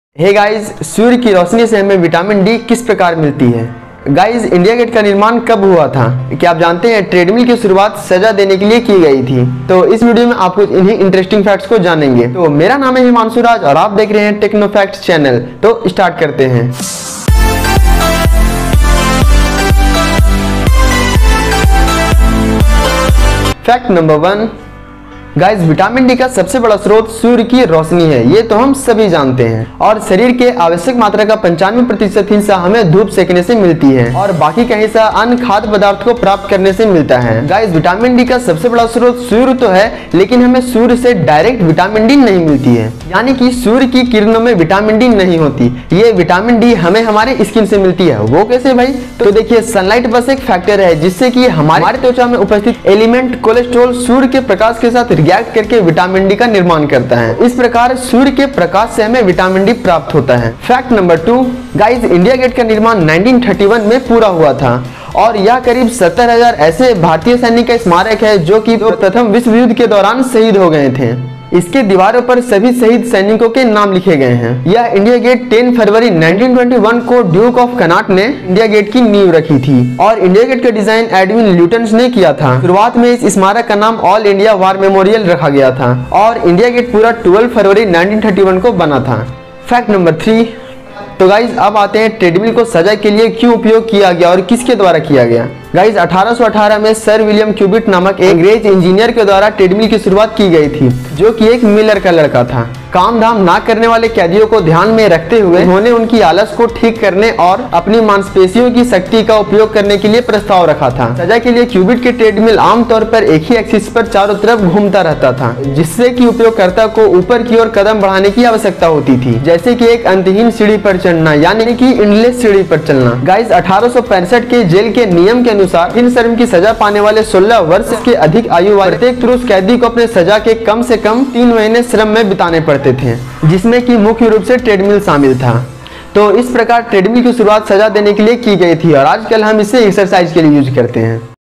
हमें Sunlight से Vitamin D कैसे मिलती हैंll How do we get Vitamin D from sunlight ?
Hey guys.

In this video I wanna tell some random facts and so many interesting facts.

1. हमें Sunlight से Vitamin D कैसे मिलती हैं ?

How do we get Vitamin D from sunlight ?

2. India Gate का निर्माण कब हुआ था ?

When was India Gate built?

3. ट्रेडमिल सबसे पहले सज़ा देनें के लिए सुरुआत हुऐ थे?

Treadmills were first introduced to punish?

This Video Is Going To Be Crazy So Watch Till End.

--- Himanshu Raj